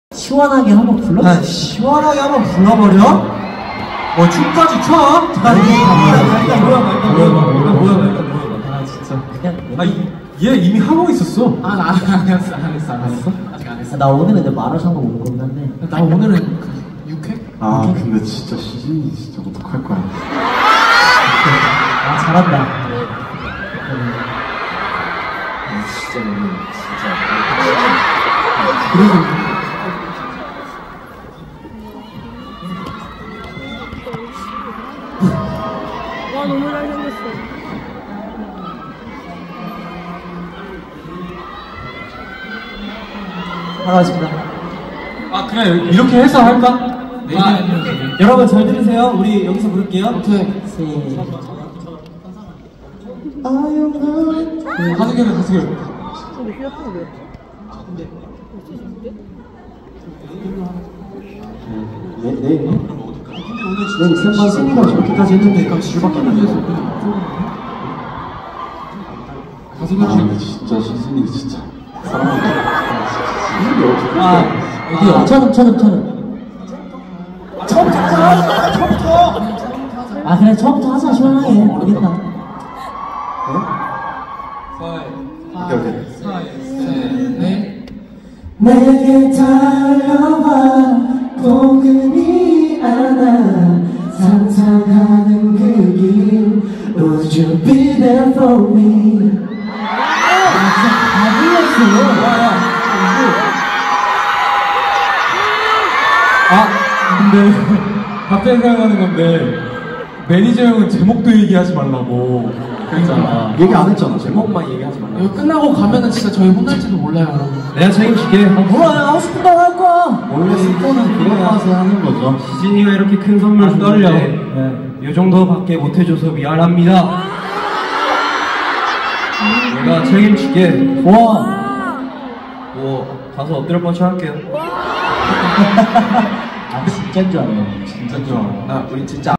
231125 NCT 127 spoil Winter Album 🖤 NCT 127 3rd TOUR NEO CITY: SEOUL "THE UNITY" DAY 5 [4K]
Zone 34, Row 04, KSPO Dome
Captured by Samsung Galaxy S23 Ultra in Pro video